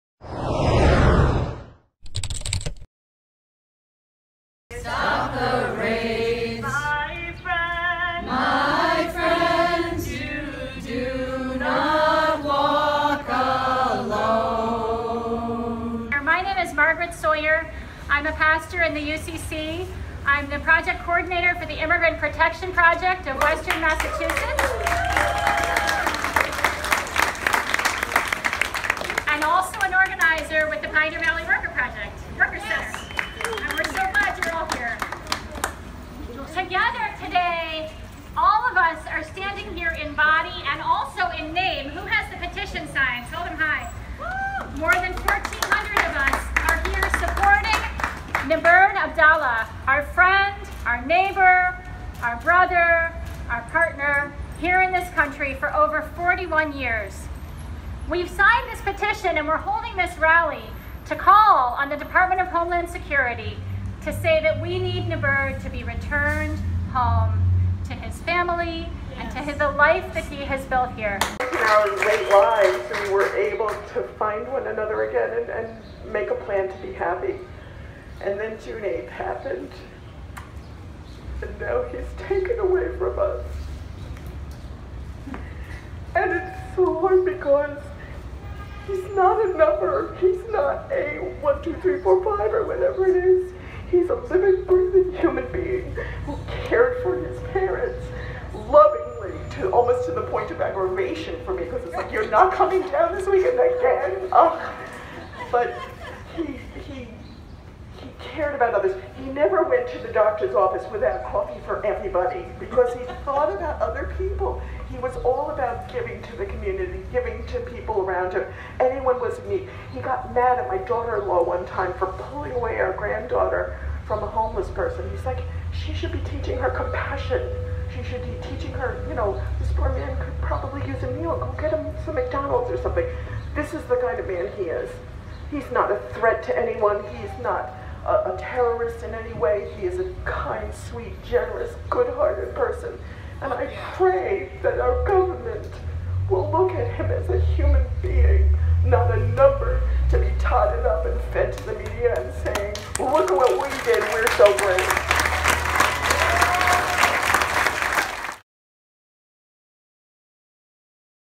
Demonstrators rally in support of Iraqi immigrant and Pioneer Valley resident facing deportation.
About 250 people, many holding signs and chanting anti-deportation slogans, rallied Tuesday in front of Northampton City Hall to protest the arrest and planned removal of longtime Pioneer Valley resident and unauthorized Iraqi immigrant Niberd Abdalla. (Don Treeger / The Republican)